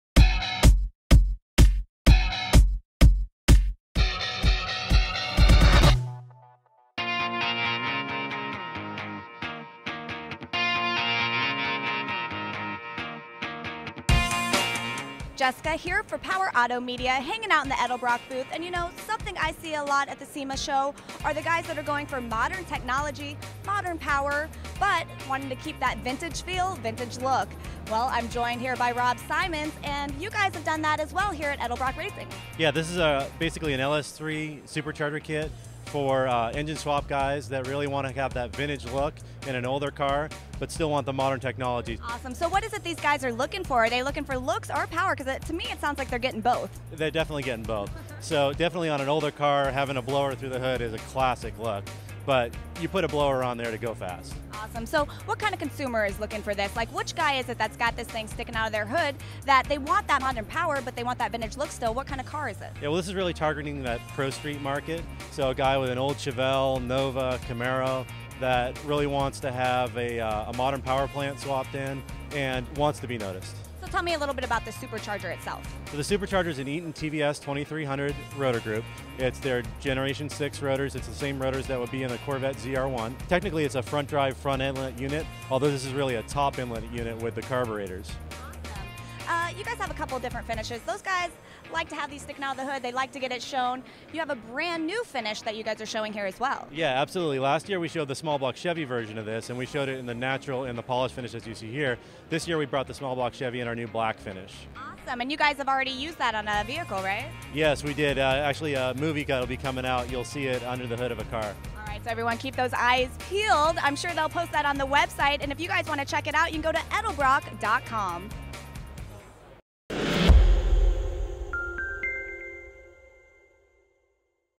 NEW - Edelbrock E-Force Street Classic LS Superchargers SEMA 2013
Introducing the NEW Edelbrock LS E-Force Street Classic Superchargers for LS Applications. Enjoy the power and performance of modern E-Force supercharging technology with classic styling cues. Nothing like a big Huffer sticking out of a muscle car hood!